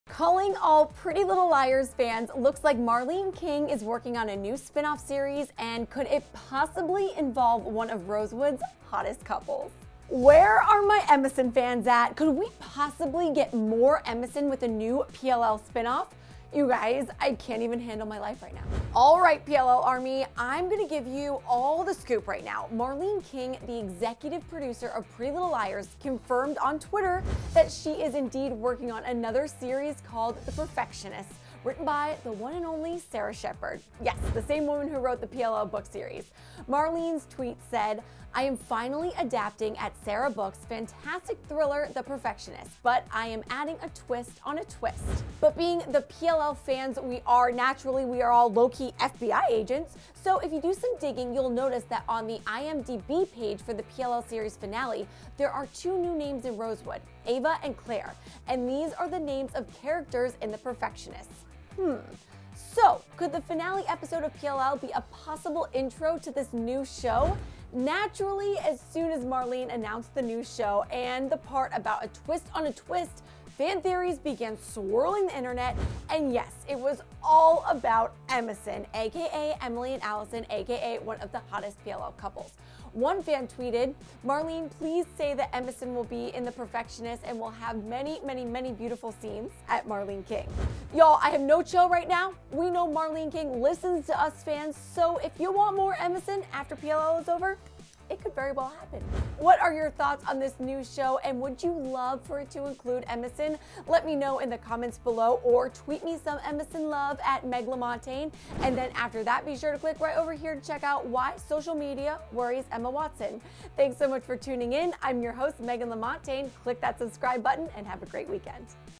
Pretty Little Liars Emison Spin-Off In The Works?
Where my Emison fans at!? Could we possibly get more Emison with a possible new PLL spin off? I can’t even handle my life right now!

Alright PLL army, I’m going to give you all the scoop I know! Marlene King, the Executive Producer of Pretty Little Liars confirmed on twitter that she is indeed working on another series called The Perfectionists witten by the one and only Sara Shepard. Yes the same woman who wrote the PLL book series.
Marlene’s tweet said, “‪@sashayfields‪ I am finally adapting @sarabooks fantastic thriller "The Perfectionist". But I am adding a twist on a twist.‬”
But being the PLL fans we are naturally we are all lowkey FBI agents, so if you do some digging you’ll notice that on the IMBD page for the PLL series finale there are two new names in Rosewood, Ava and Clair, and these are the names of characters in The Perfectionists. Hmmmm. So could the finale episode of PLL be a possible intro to this new show?
Naturally, as soon as Marlene announced the new show and the part about a twist on a twist fan theories began swirling the internet! And yes it was all about Emison! Aka Emily and Alison, aka one of the hottest PLL couples!
One fan tweeted, “Abby ‪@Cece_is_A
Marlene please say that Emison will be in the perfectionists and will have many many many beautiful scenes ‪@imarleneking”
Ya’ll I have no chill right now. WE know Marlene listens to us fans so if you want more Emison after PLL is over, it could very well happen!
What are your thoughts on this new show and would you love for it to include Emison? And then after that be sure to check out why social media worries Emma Watson right over here. Thanks so much for tuning in!